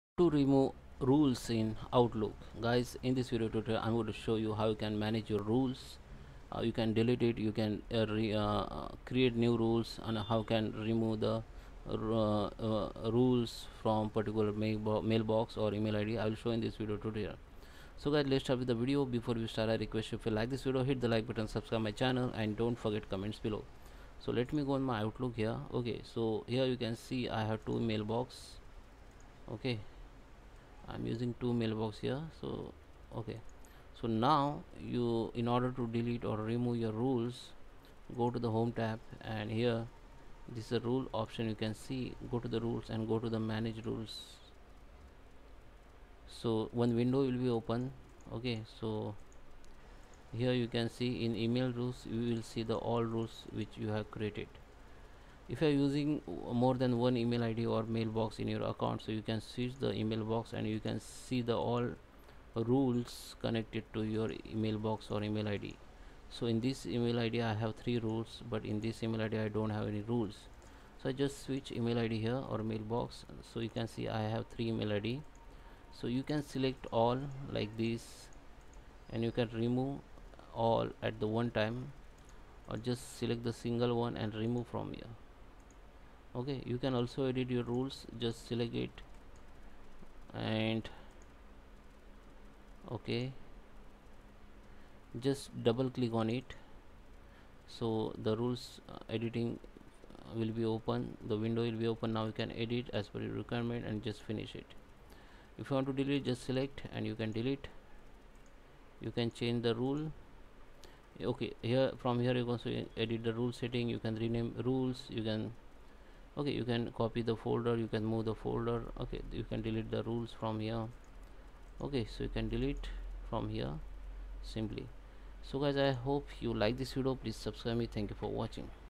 How to Remove Rule Outlook | How to Remove All Outlook Rules
In this video tutorial, we will learn how you can remove rule outlook and  how to remove, edit all outlook rules.